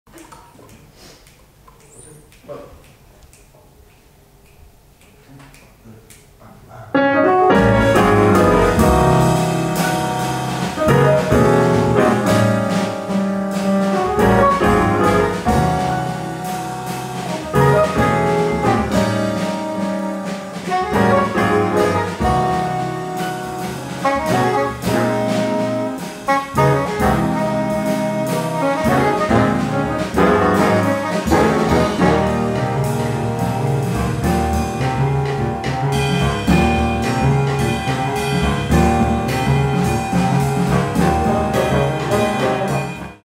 Ensaio: OJM & João Pedro Coelho (Novos Talentos do Jazz)
Ensaio no estúdio da Orquestra Jazz de Matosinhos para o concerto de 1 de Março 2019, com o pianista João Pedro Coelho, convidado integrado no ciclo Novos Talentos do Jazz.